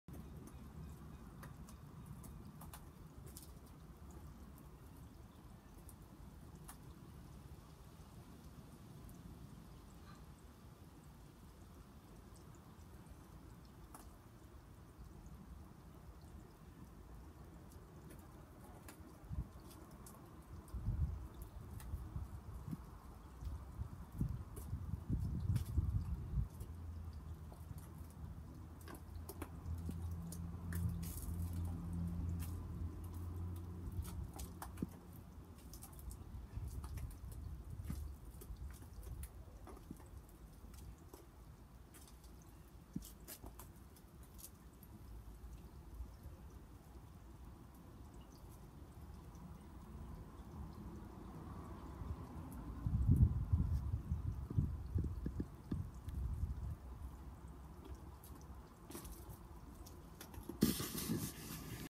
Polkadot Dress in Garden
Johanna Clayton poses in garden sunshine.